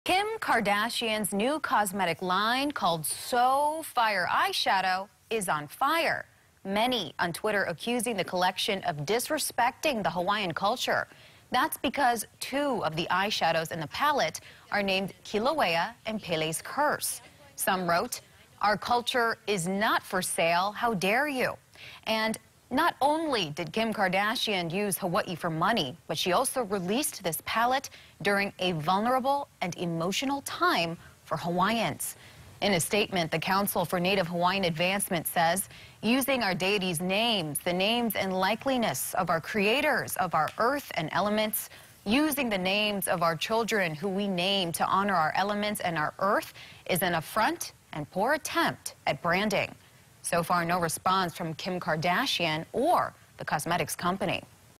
Kim Kardashian's eyeshadow upsets Native Hawaiians
Two of the eyeshadows in the palette are named Kilauea and Pele's curse.